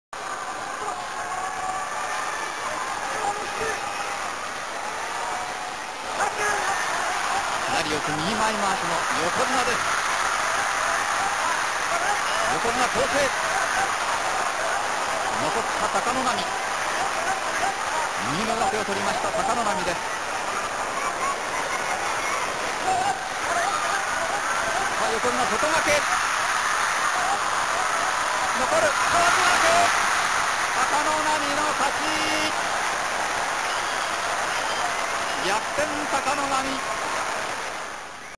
Takanonami vs. Takanohana : Hatsu 1996 (貴ノ浪 対 貴乃花)
Kettei-sen de l'Hatsu 1996 : Membres de la même Heya, l'Ozeki Takanonami et le Yokozuna Takanohana s'affrontent pour le Yusho. Les deux lutteurs avaient terminé à égalité avec 14 victoires.
Takanonami l'emporte par Kawazugake. C'est le premier des deux Yusho remportés par l'Ozeki dans sa carrière.